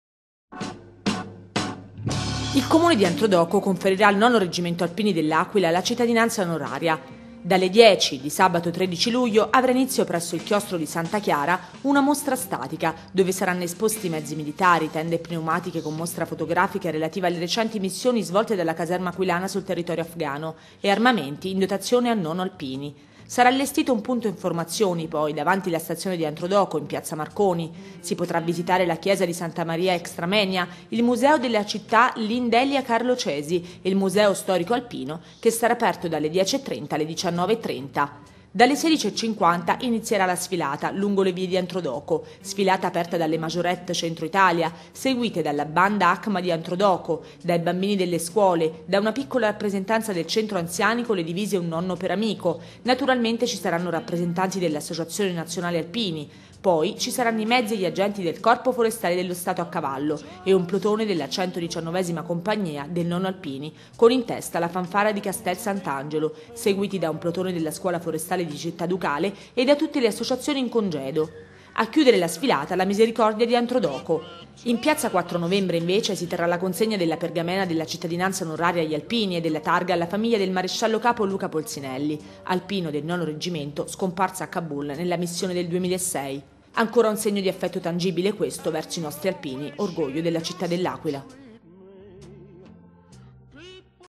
ANTRODOCO CONFERISCE LA CITTADINANZA AGLI ALPINI AQUILANI
Il comune di Antrodoco conferisce la cittadinanza onoraria agli alpini del Nono dell'Aquila in una cerimonia che avrà luogo sabato 13 Luglio. Una intera giornata che vedrà la mattina una mostra sulla recente missione in terra afghana dei nostri alpini con le foto scattate durante la missione dal fotografo del Nono Daniele Di Benedetto. Il pomeriggio la sfilata per le vie del centro storico con un plotone della 119 compagnia del Nono Alpini